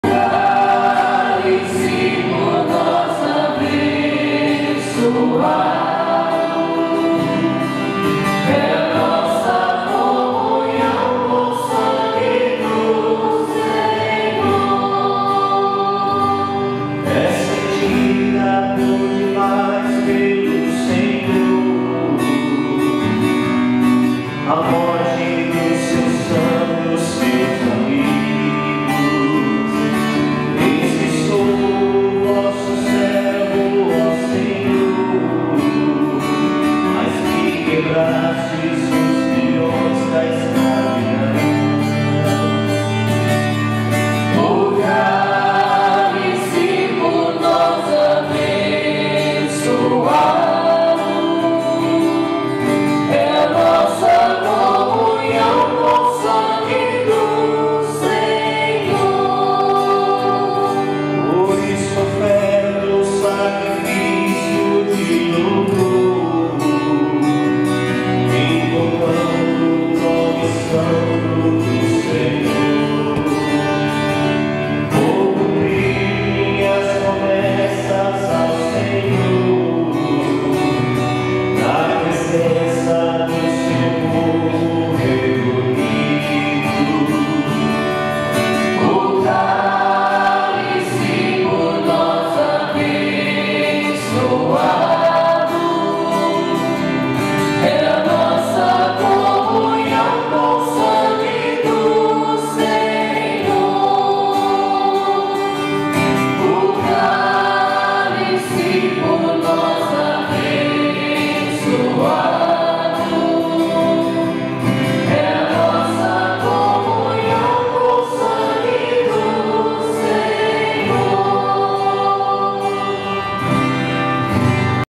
Santa Missa do Lava Pés e instituição da Eucaristia
A Paróquia Nossa Sra. Aparecida de Barra do Jacaré, Presidida pelo Padre Claudio Leite na noite de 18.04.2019 veio a realizar a Santa Missa do Lava pés e da Instituição da Eucaristia. Se apresentando durante a Missa o seu Grupo de Cânticos composto por: Augustinho (Tim), Sônia Peres; Fernanda Calixto, Julcilêa Dutra e Fernando Trevisan, cantando lindas músicas de louvores a Deus .